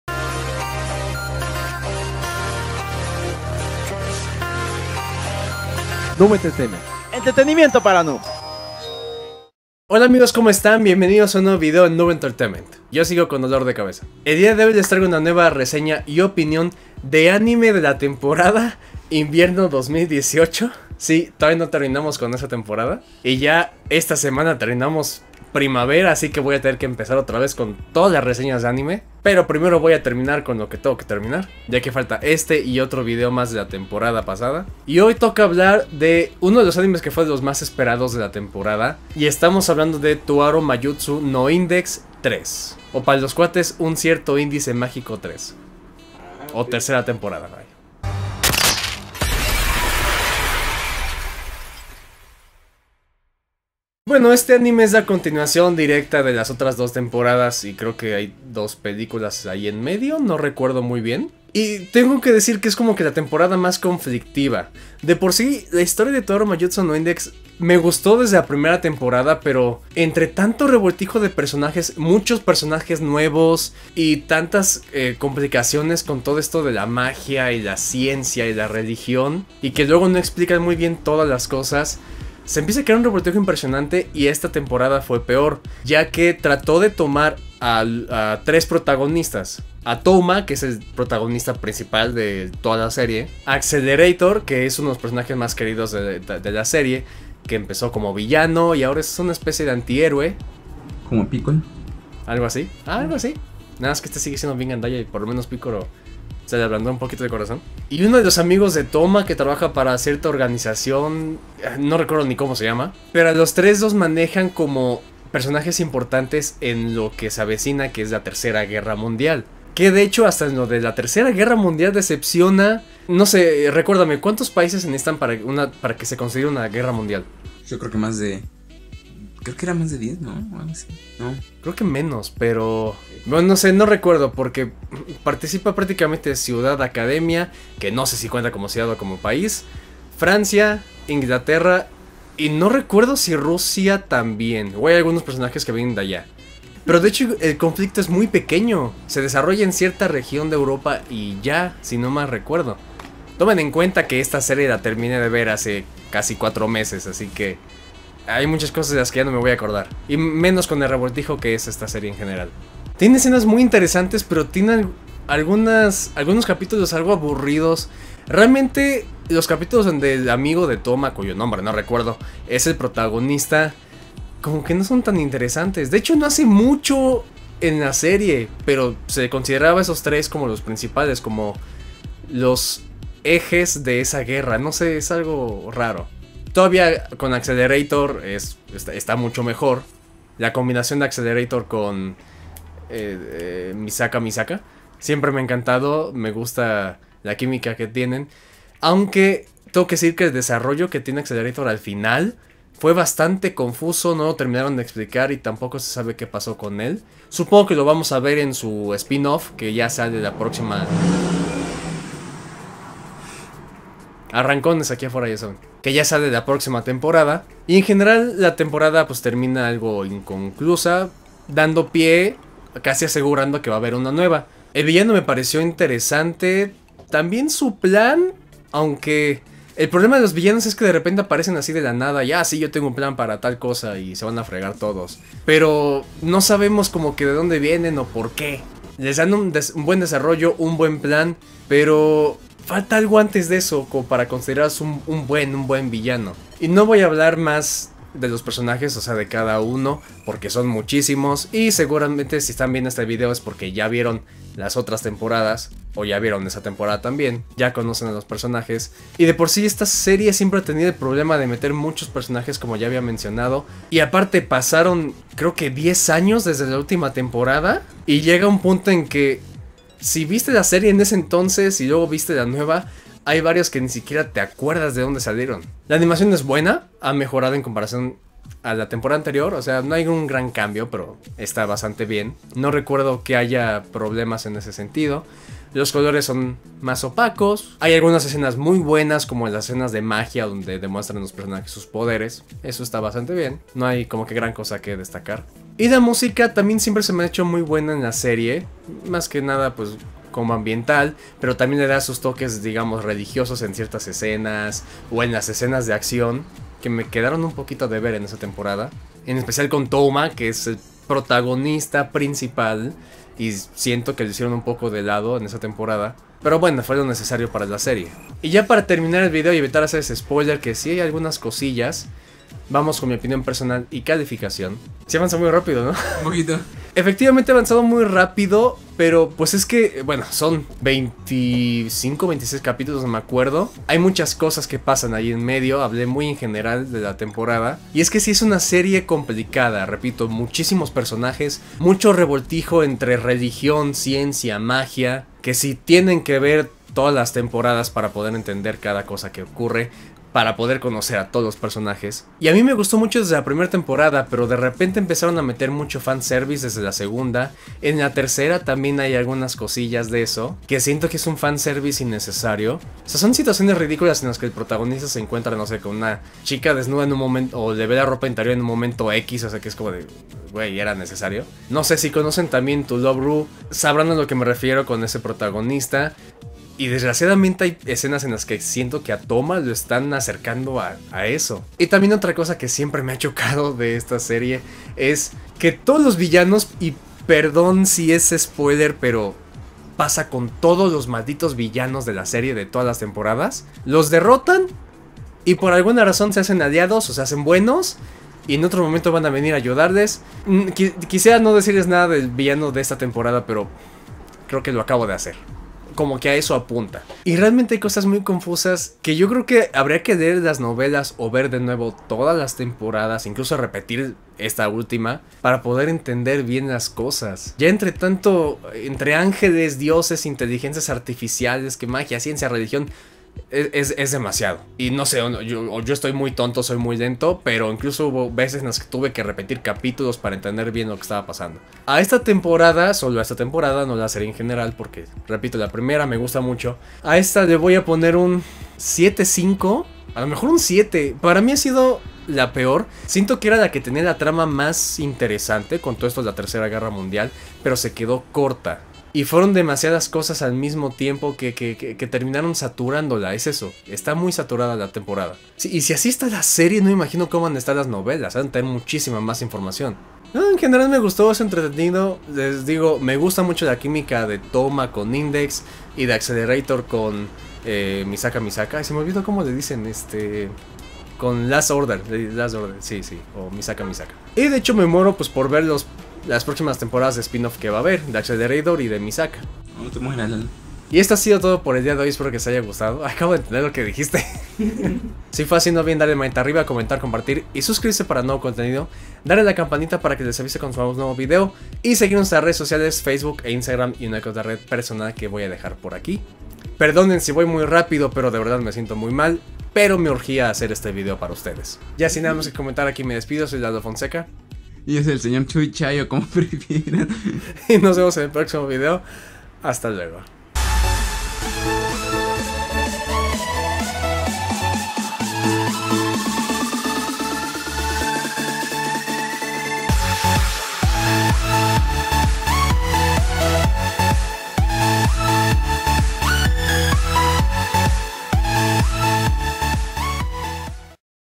Toaru Majutsu No Index III (A Certain Magical Index III) - Reseña y Opinión
¡Bienvenidos a un nuevo video! En esta ocasión les traemos nuestra reseña y opinión (sí, continuamos con la temporada Invierno 2019) de "Toaru Majutsu No Index III", continuación de la serie de ciencia, religión, poderes y magia que vuelve luego de varios años.